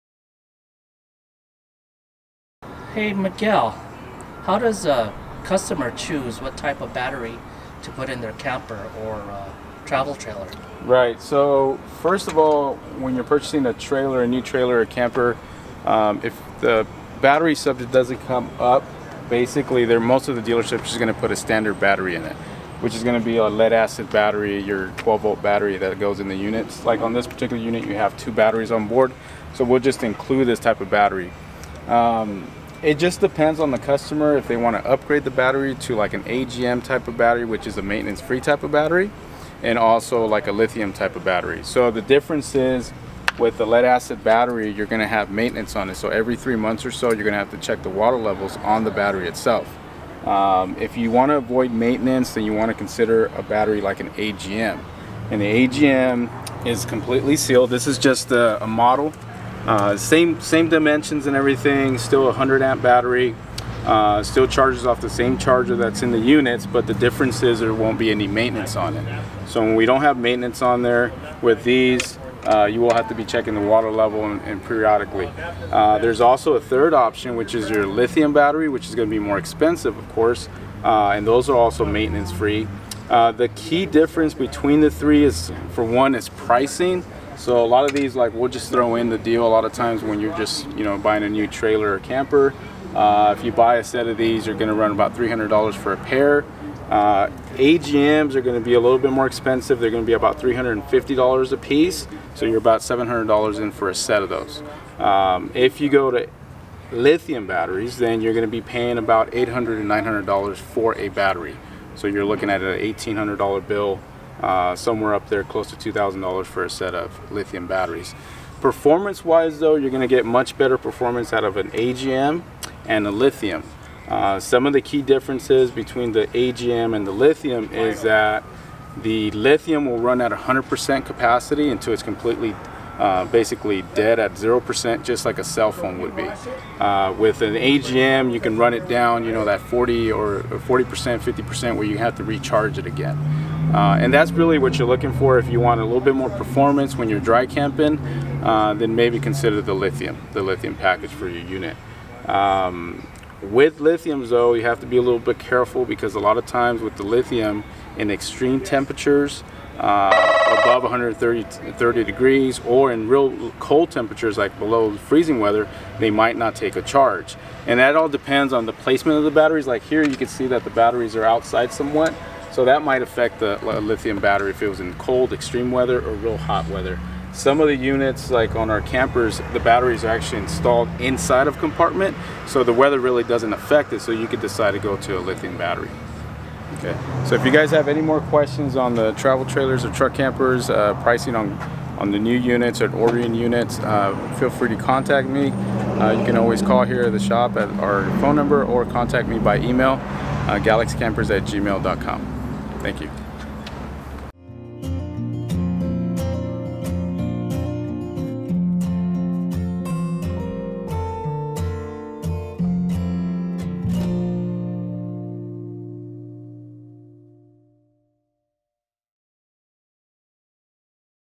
Best Battery for your RV or Travel Trailer
Miguel explains the difference between the different types of batteries that your RV, Camper or Travel Trailer can use. He explains the cost, benefits and downfall of each. I hope this video helps you in deciding what's best to fit your needs!

See the full inventory of Lance Campers and Travel Trailers at:
Galaxy Campers